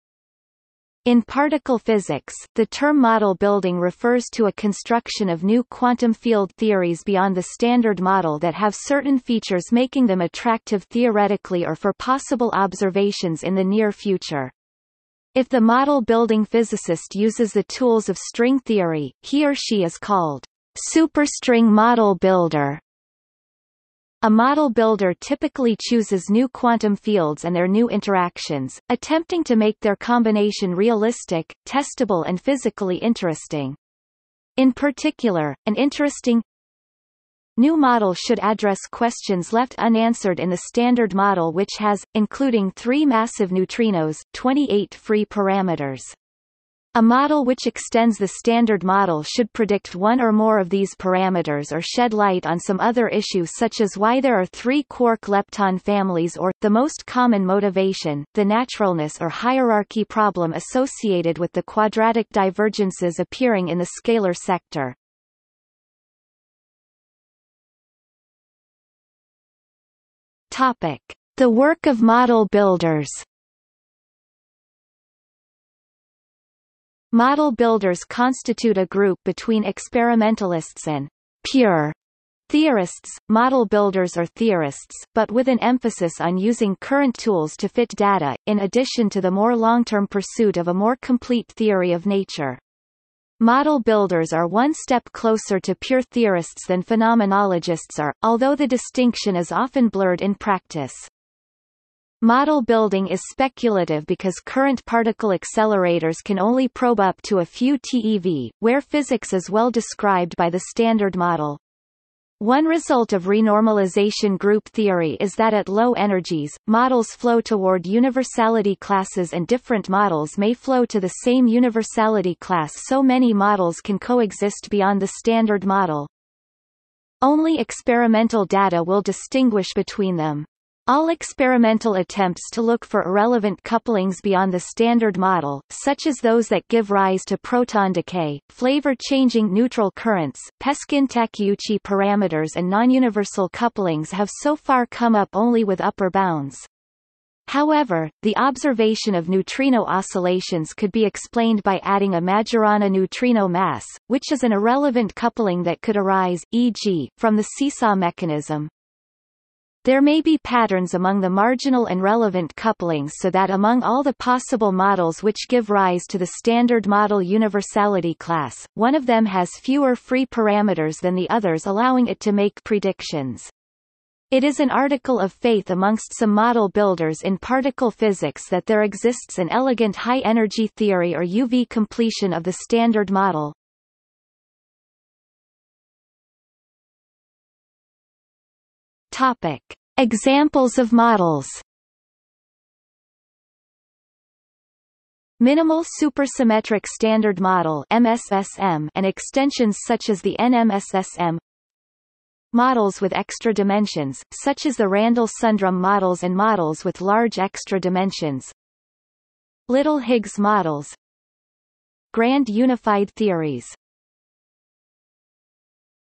Model building (particle physics) | Wikipedia audio article
00:01:06 The work of model builders
00:03:05 Examples of models

- Socrates

SUMMARY
In particle physics, the term model building refers to a construction of new quantum field theories beyond the Standard Model that have certain features making them attractive theoretically or for possible observations in the near future. If the model building physicist uses the tools of string theory, he or she is called "superstring model builder".
A model builder typically chooses new quantum fields and their new interactions, attempting to make their combination realistic, testable and physically interesting. In particular, an interesting
new model should address questions left unanswered in the Standard Model which has, including three massive neutrinos, 28 free parameters. A model which extends the Standard Model should predict one or more of these parameters or shed light on some other issue such as why there are three quark-lepton families or, the most common motivation, the naturalness or hierarchy problem associated with the quadratic divergences appearing in the scalar sector.